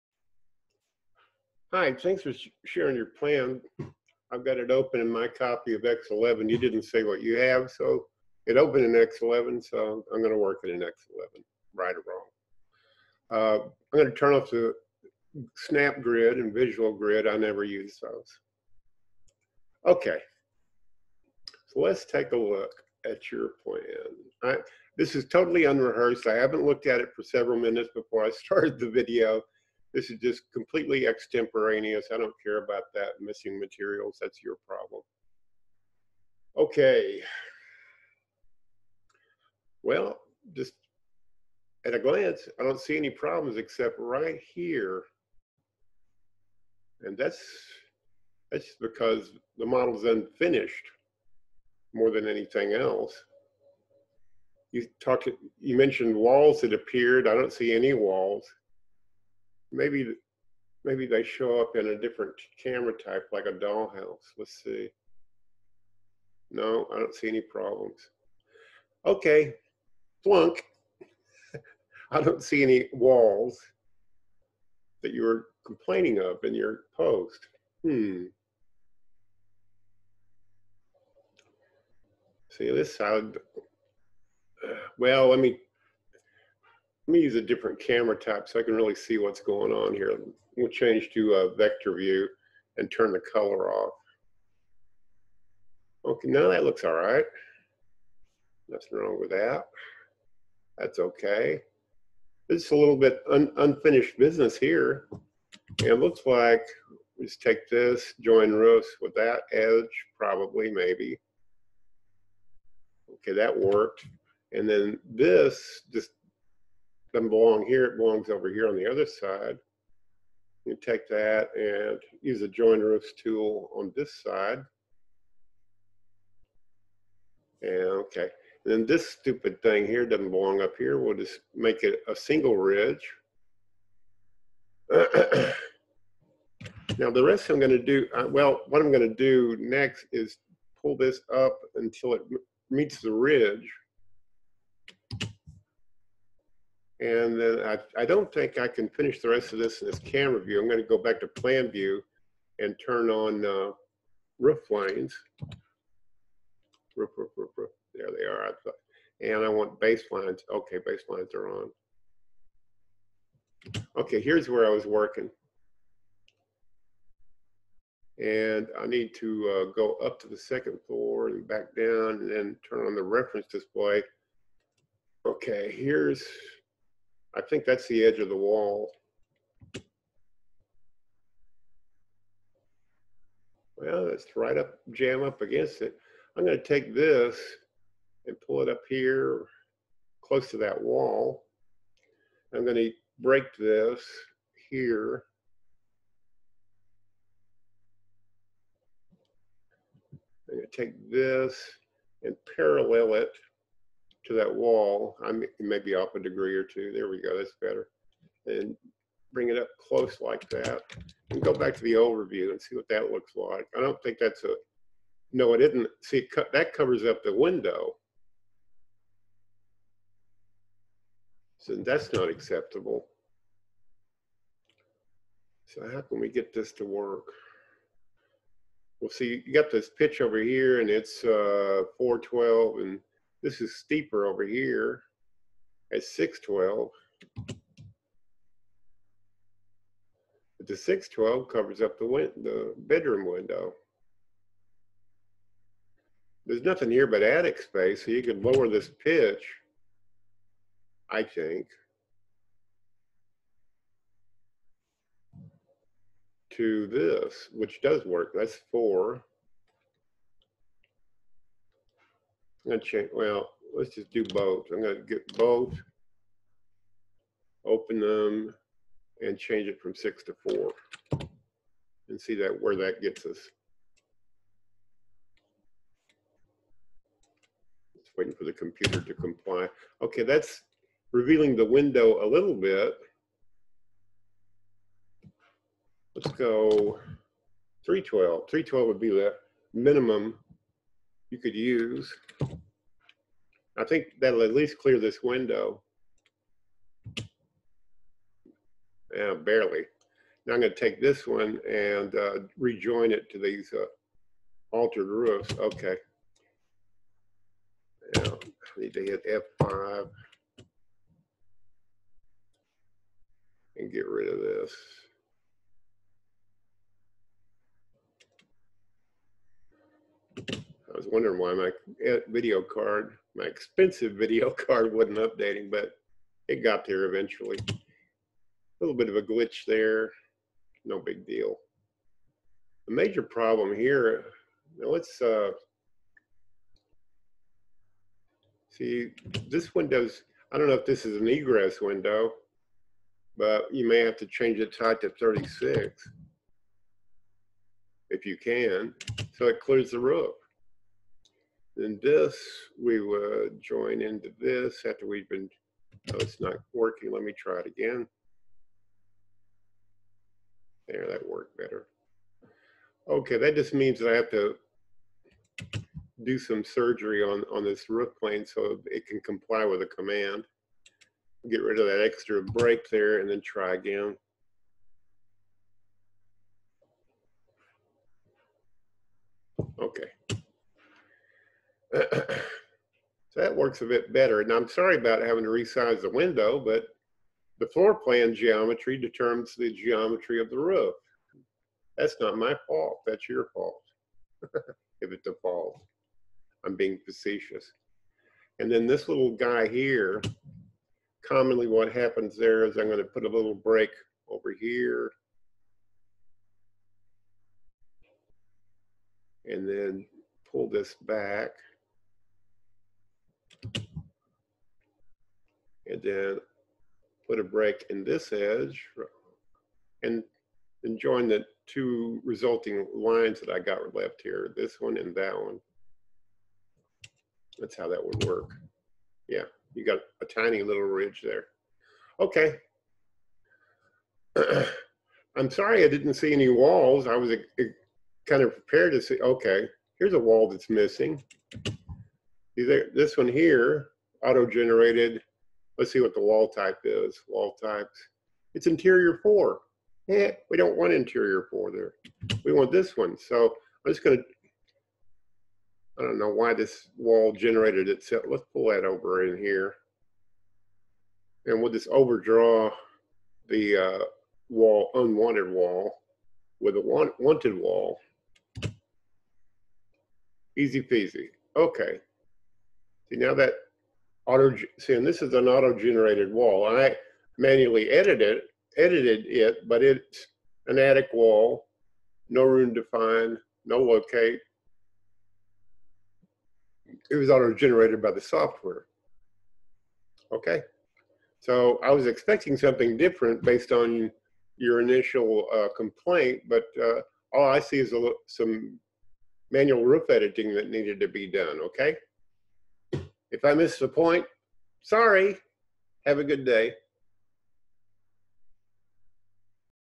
Chief Talk problem
Said the problem was with auto-generated walls but I found it was mainly a roof problem